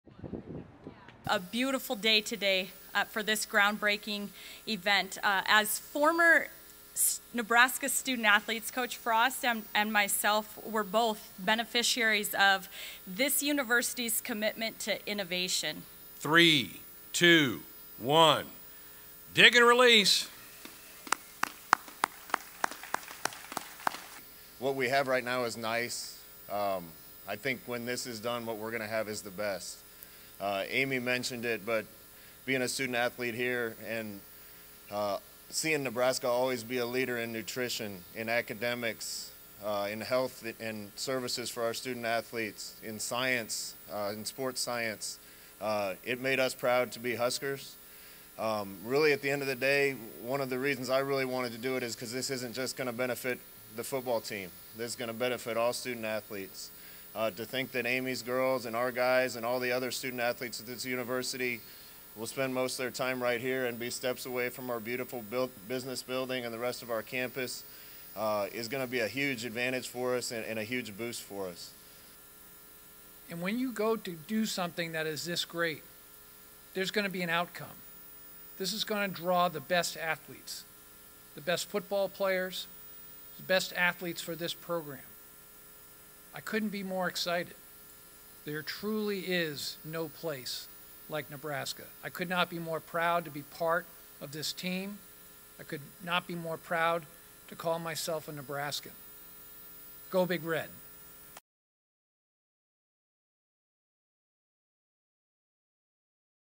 Nebraska finally breaks ground on new football facility
Nebraska finally breaks ground with new $155 million football facility